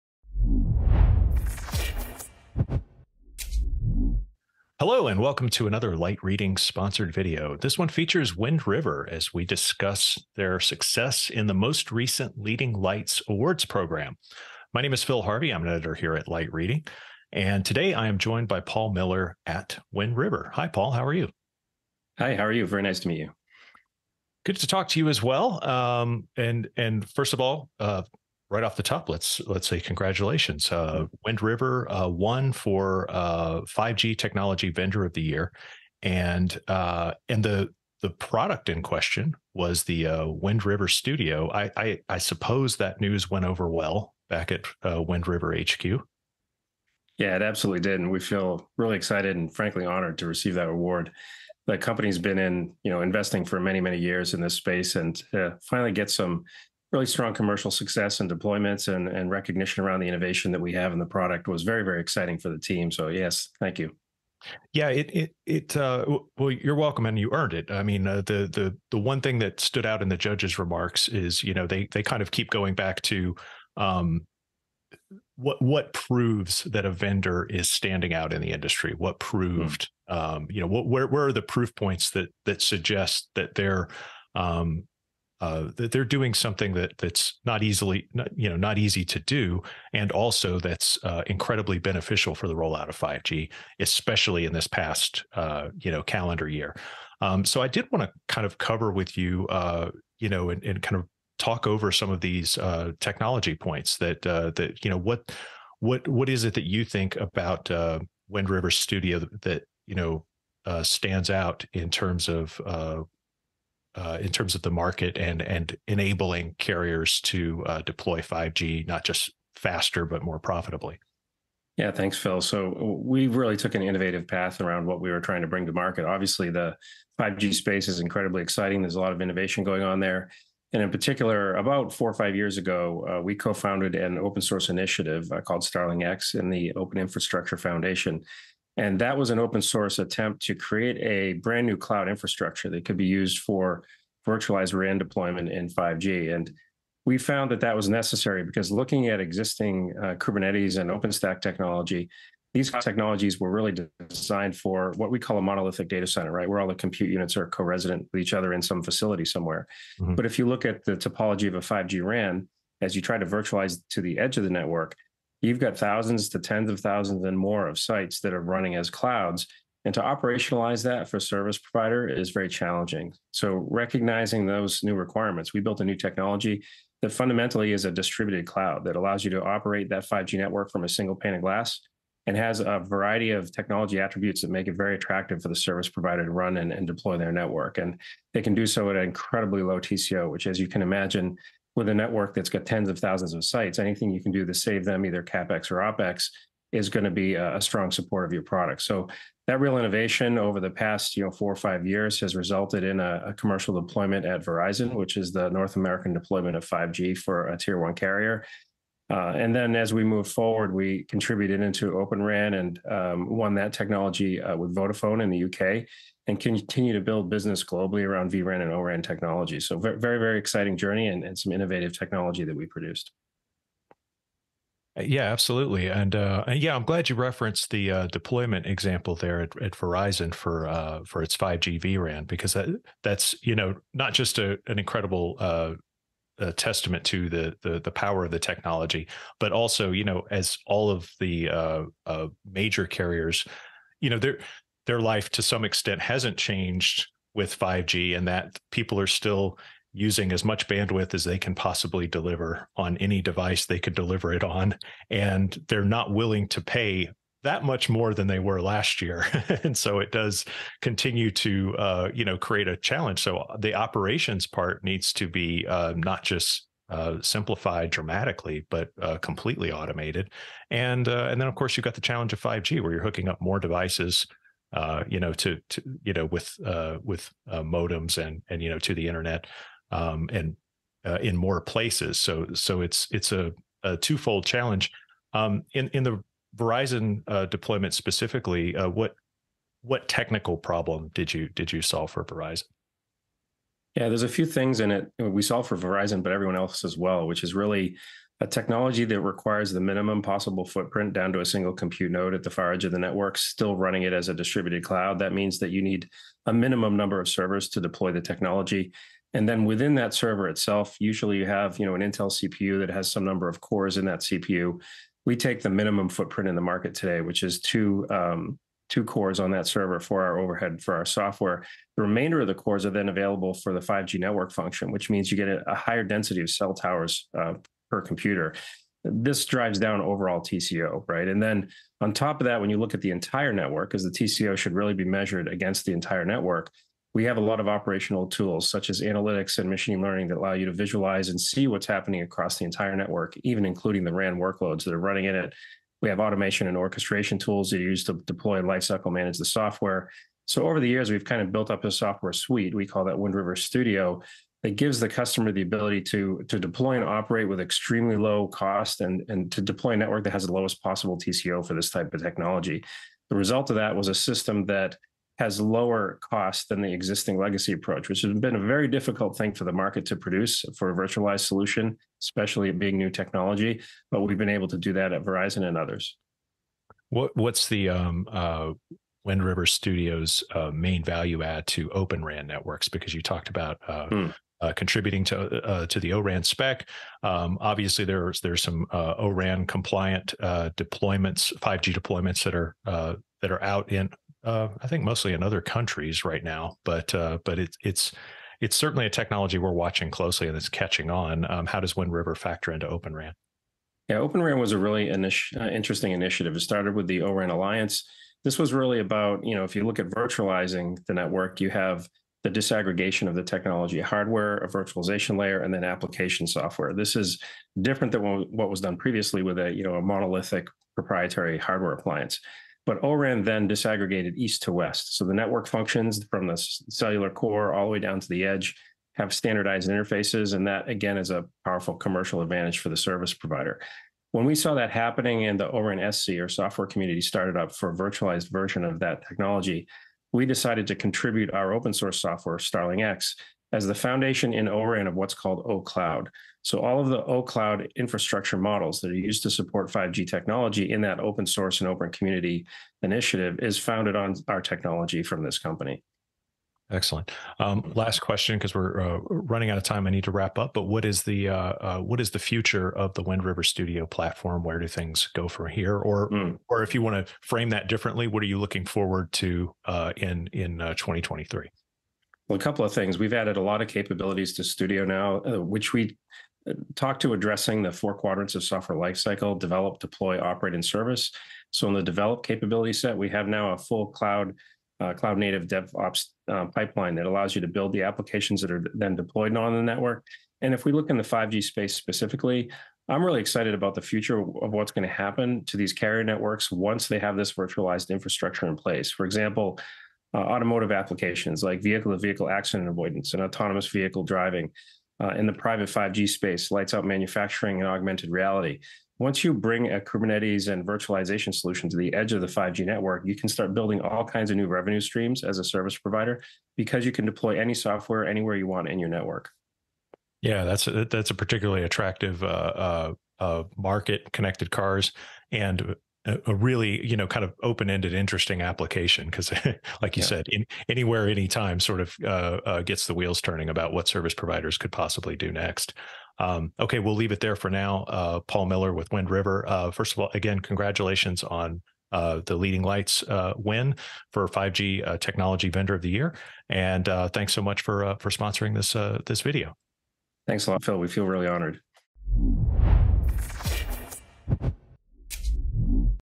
Wind River - Winner of the Light Reading Leading Lights award for 5G Technology Vendor of the Year
Phil Harvey speaks with Wind River CTO Paul Miller about some of the reasons Wind River Studio won the Leading Lights 2022 award for "5G Technology Vendor of the Year." They delve into topics such as virtual and open RAN, open source StarlingX, ...